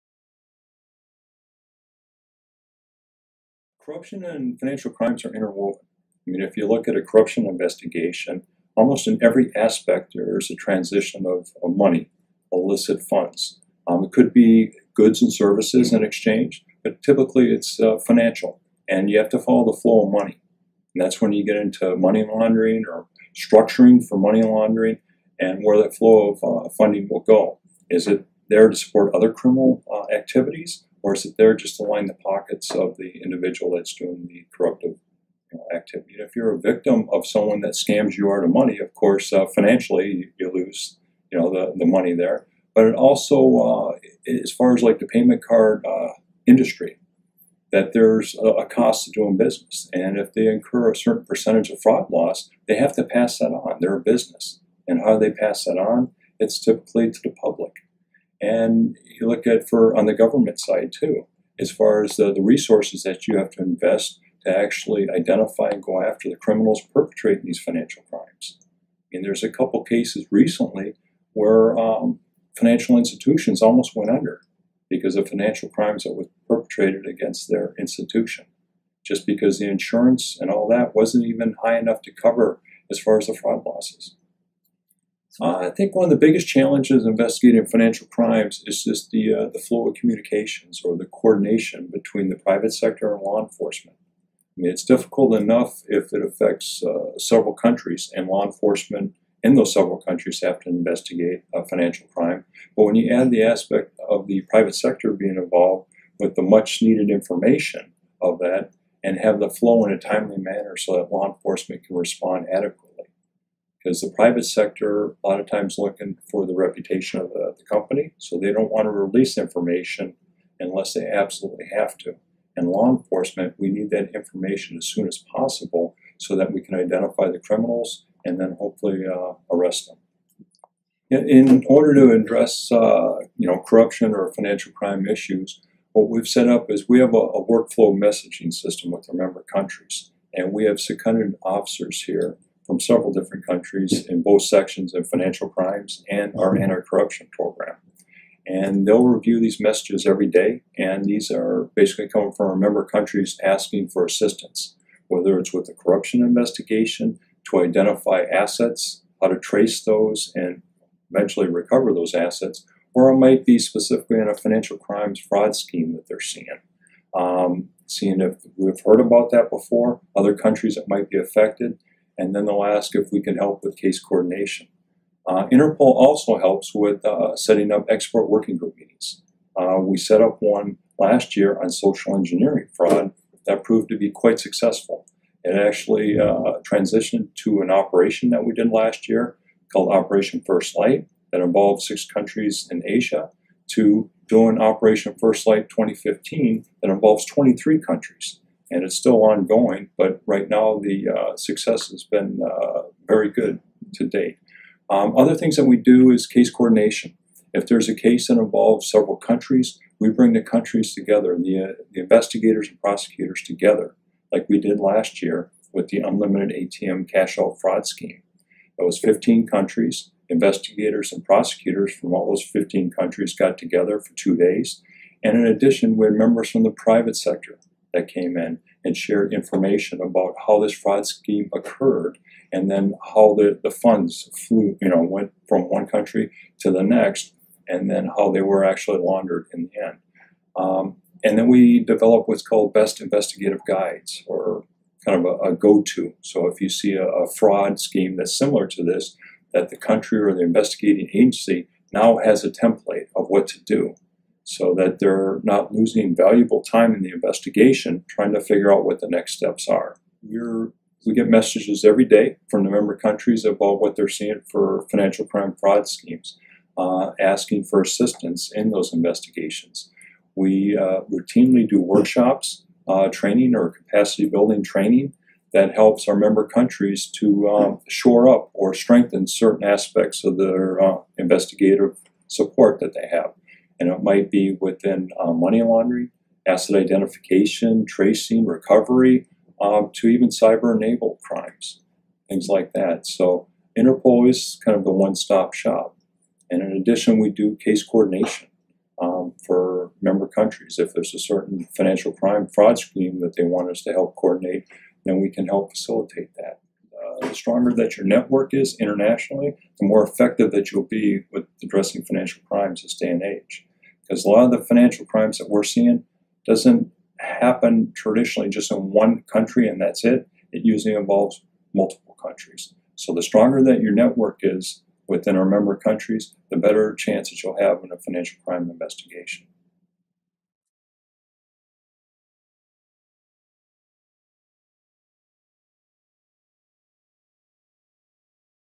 James ANDERSON, INTERPOL Assistant Director Anti Corruption and Financial Crime Sub Directorate
The effects of corruption are multi-dimensional, with consequences in political, economic and social aspects of life at all levels and in all countries.

Corruption undermines the legitimacy of governments and creates a fertile ground for organized crime and terrorism, potentially de-stabilizing communities.

Corruption is often responsible for

• The diversion and misallocation of resources from much needed projects such as schools, hospitals or roads;

• conversion of public wealth to private and personal property;

• inflation;

• imbalanced economic development;

Illegally obtained funds from corruption are also laundered and moved around the world through a variety of methods to avoid detection, often before being re-integrated into legitimate businesses and the economy.

Private individuals, companies, organizations and nations are all affected by financial crime, with the Internet facilitating the ease with which these crimes are committed, often in a country far away from where the offender logs in.

At INTERPOL, we work to combat all financial crimes through the global exchange of information, supporting operations in the field, and bringing together experts from the sectors concerned.

We work closely with regional and international organizations to identify trends and responses and to avoid duplication of effort, which is essential in dealing with financial crimes where time is of the essence.

Law enforcement can only take action within a strong legal framework, not just to identify and arrest those responsible, but also for the recovery of assets illegally taken from public purse.

INTERPOL plays a unique role in assisting its 190 member countries address these truly international challenges. Its global specialized tools and network can identify links which may otherwise go unnoticed, and more importantly help law enforcement on the ground locate criminals who may otherwise remain at large.